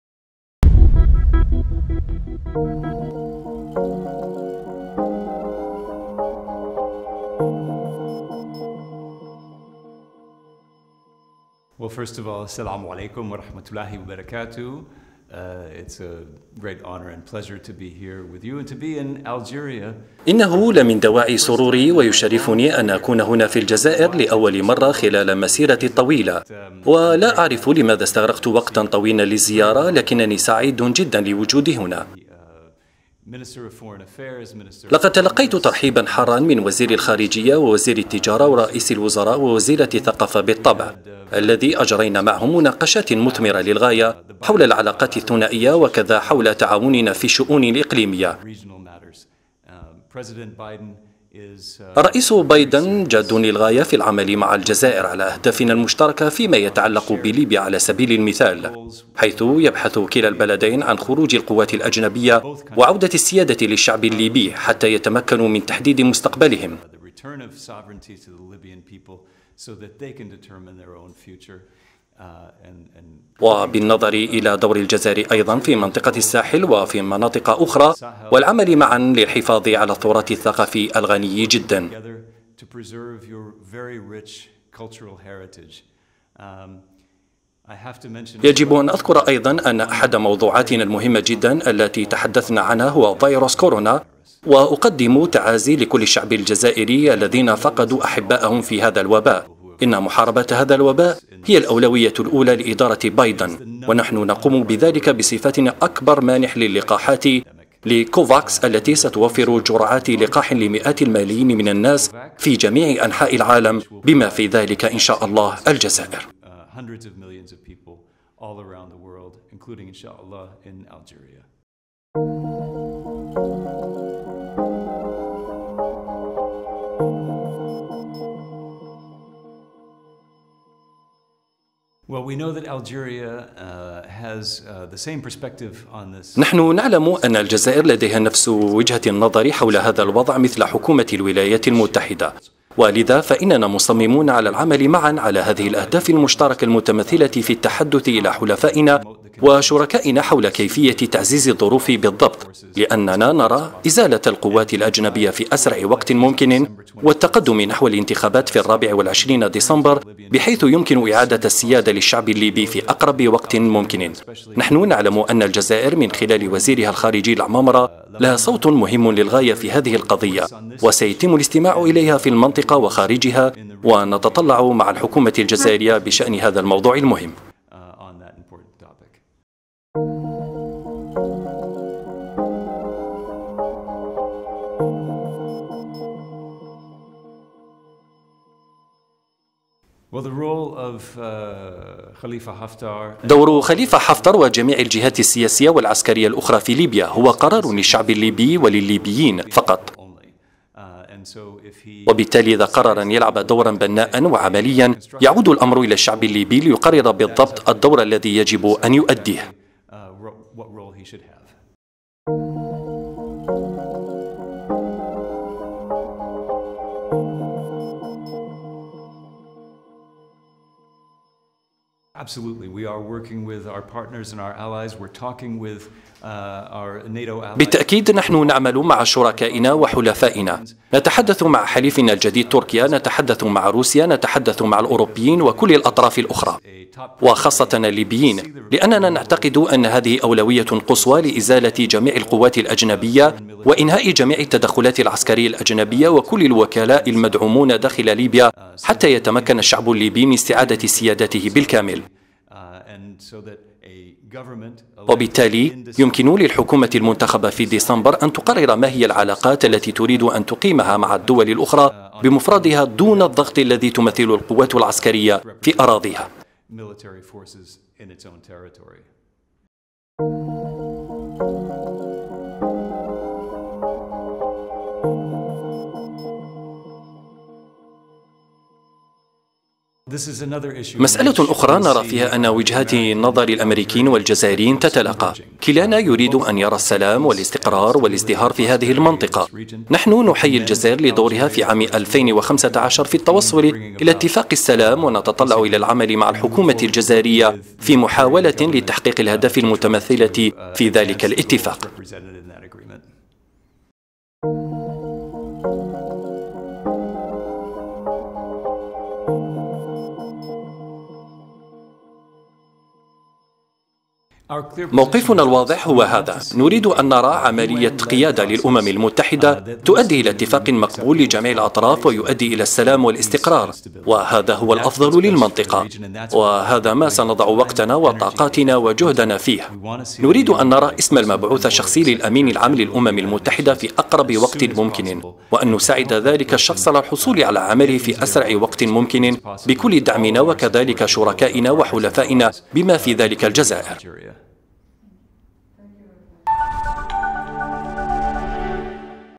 الحوار الخاص لمساعد وزير الخارجية الأمريكية بالنيابة مع وكالة الأنباء الجزائرية
الجزائر (وأج )-  أكد مساعد كاتب الدولة الأمريكي المكلف بالشرق الأوسط, السيد جوي هود, في لقاء خص به (وأج) على هامش زيارة العمل التي قام بها الى الجزائر في الفترة (25 و 26 يوليو الجاري),تقارب وجهات النظر بين الجزائر والولايات المتحدة الامريكية فيما يتعلق بتسوية العديد من الازمات.